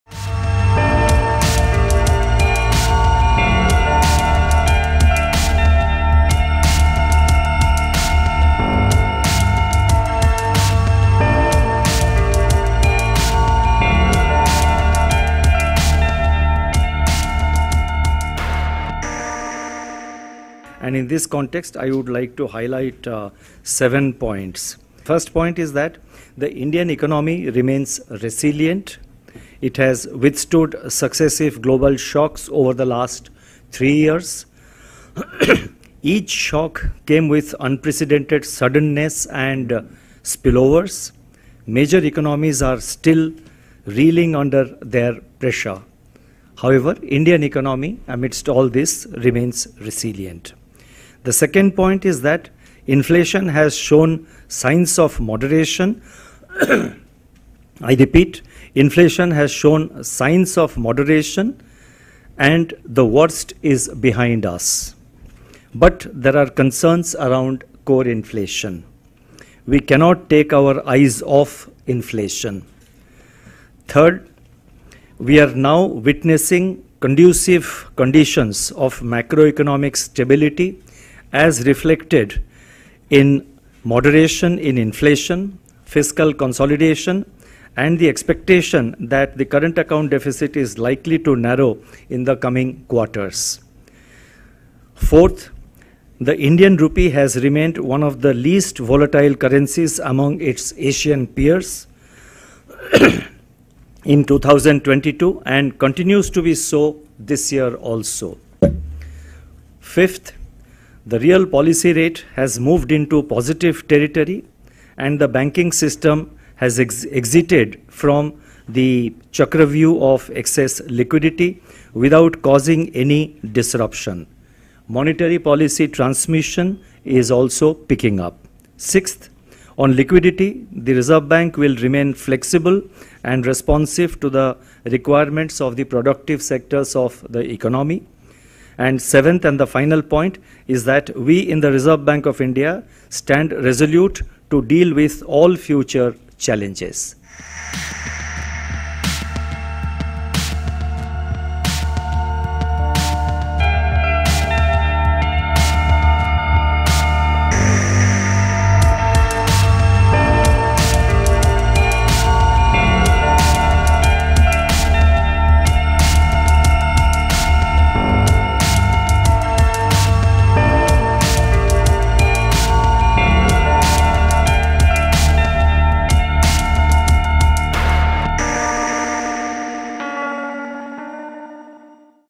From state of inflation to health of banks I All you need to know about the monetary policy changes
After a bi-monthly MPC or monetary policy committee meeting held between 6th-8th Feb RBI Governor announced the monetary policy today. Here are 7 things you need to about it.